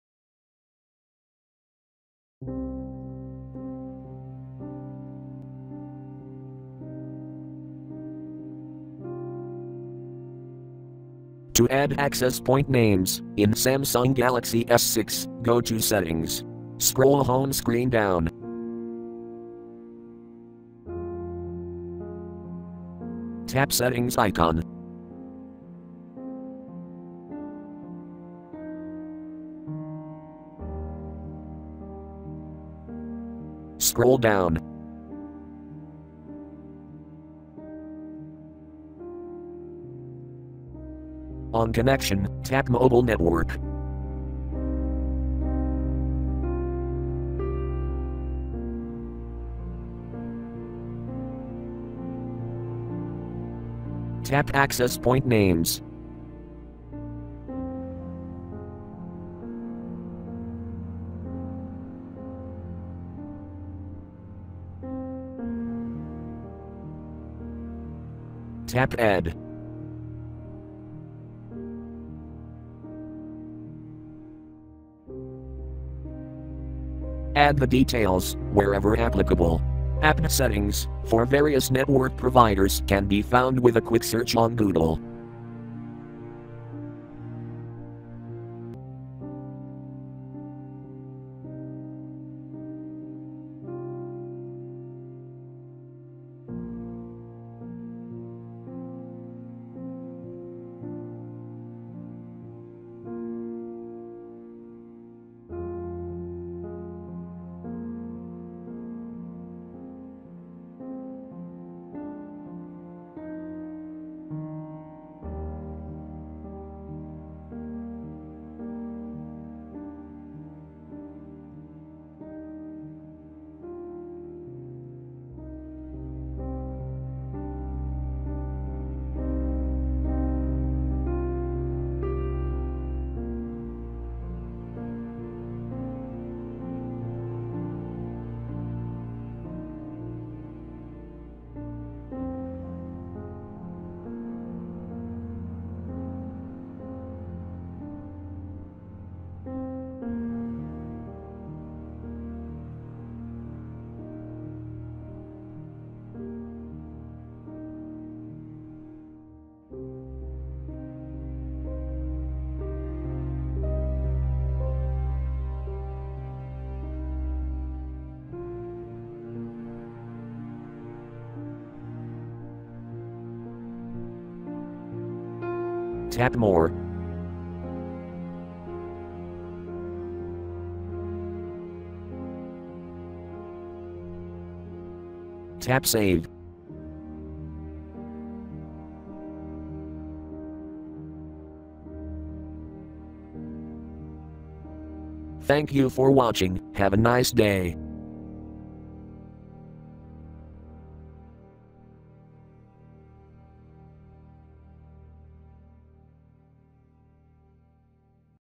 Samsung Galaxy S6 Tips : How to add APN Access Point Names
This video show How to add APN Access Point Names in Samsung Galaxy S6 or S6 Edge.  In this tutorial I use Samsung Galaxy S6 Edge SM-G925F International version with Android 5.1 (Lollipop).